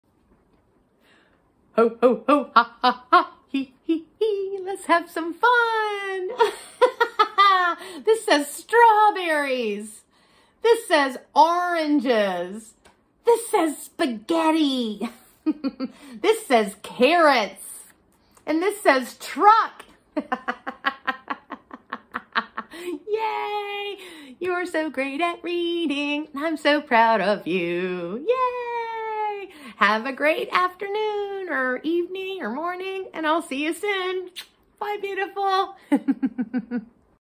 Lesson 28D Baby/Children Learning to Read English
A new 5 Days of introducing more fun words EACH DAY! Keep showing up!
Day 4 of 5 for these word sets continuing to introduce a NEW word EACH day and retiring an old word each day:  Today includes six short videos (to watch throughout the day) with two sets of five words each (with one new word in each set) but presented in a different order each time. Let your baby/toddler/child watch these super quick videos (throughout the day, but only when they are feeling HAPPY and with no other distractions/noise. (If they are teething or feeling cranky wait until they are feeling happy again. Skip a day if need be and then come back to it. Keep showing up when they are happy and stay consistent with the daily, quick, but joyful exposures to the words.) Let them just watch one at a time and then do something else and then let them watch another throughout the day - We'll review these words over the next five days and will add a new word each day until it is a new set of words. We’ll continue this pattern adding two new words daily because your baby will have learned to read them. NEVER test your baby. Just trust that they are learning. No stress, no pressure, JUST JOY! You'll be amazed how quickly they will learn to read. Babies learn so fast! NOTE TO PARENTS: At the end of each session:  TELL your child that they are very good and very bright.  TELL your child that you are very proud of him/her. TELL him that you love him/her very much. It is wise to hug your precious little one to express your love for him/her physically.